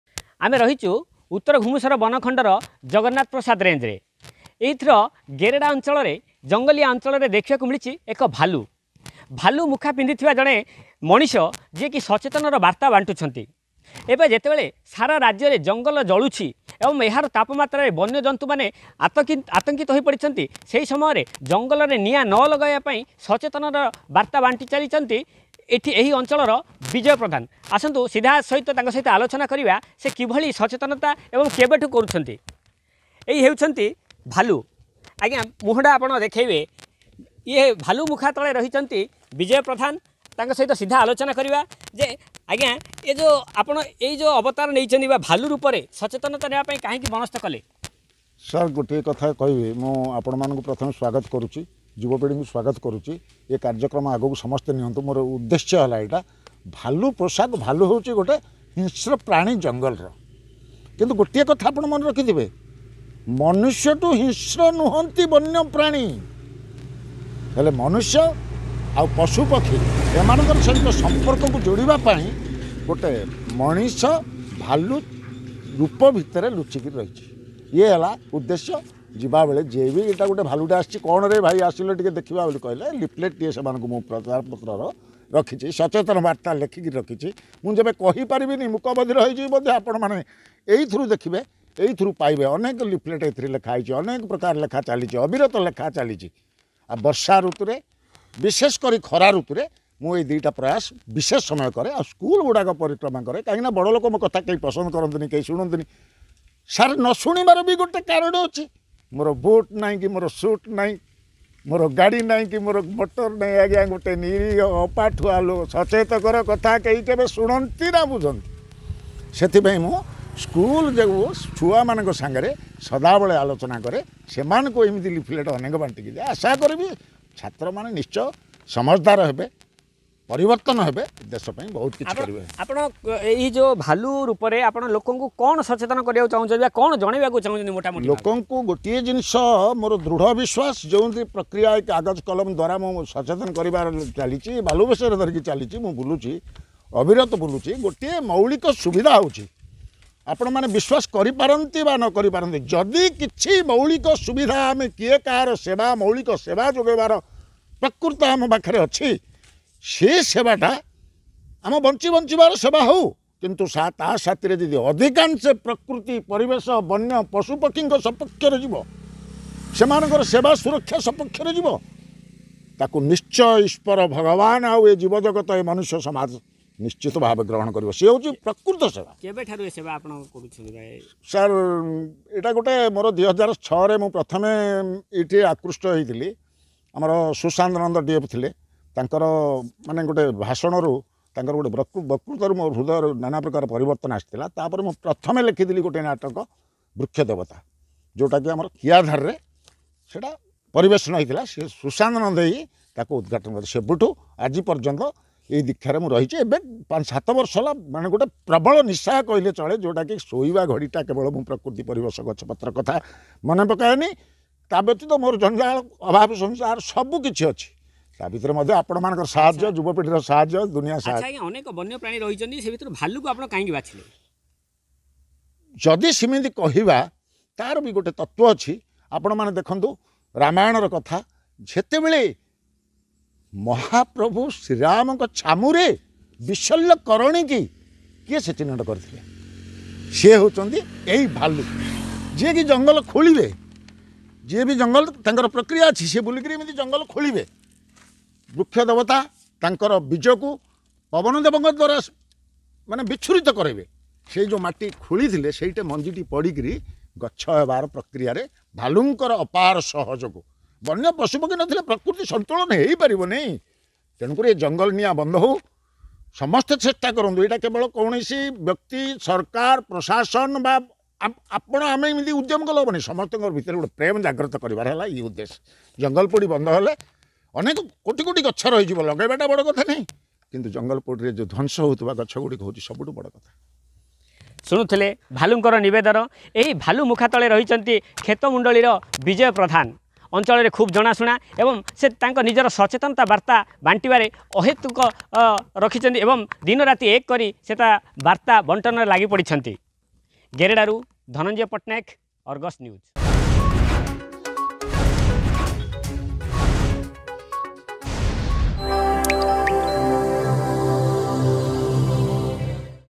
Man creates awareness wearing bear costume
ଭଂଜନଗରରେ ଭାଲୁବେଶ ପରିଧାନ କରି ଜଙ୍ଗଲରୁ ନିଆଁ ଲିଭାଇବା ପାଇଁ ଓ ନିଆଁ ନଲଗାଇବାକୁ ନିଆରାସଚେତନ କରୁଥିବା ବିଜୟ ପ୍ରଧାନଙ୍କ ସହ ଆଲୋଚନା...